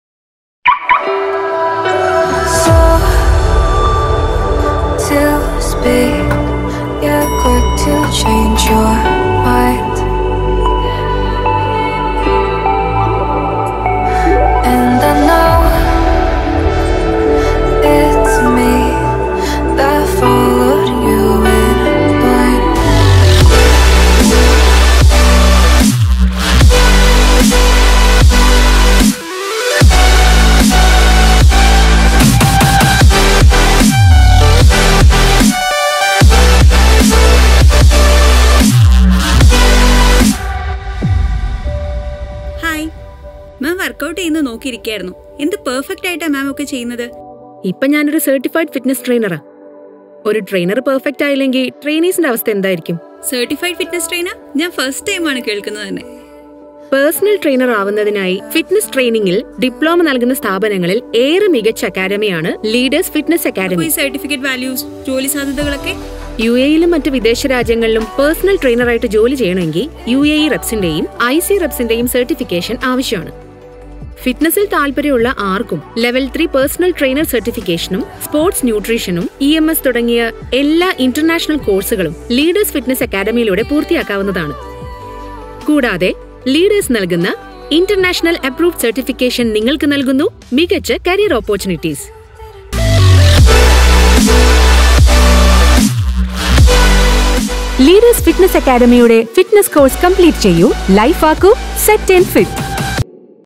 LEADERS FITNESS GYM & ACADEMY AD | SREEYA IYER | MODERNLY EQUIPPED GYM | AJ VISUALIZER
Leaders Fitness gym is modernly equipped dedicated to creating a strong community in UAE that believes in health and fitness, which is affordable and accessible to everyone, Beginners are welcome to learn basic appropriate structure and strategy from skilled trainers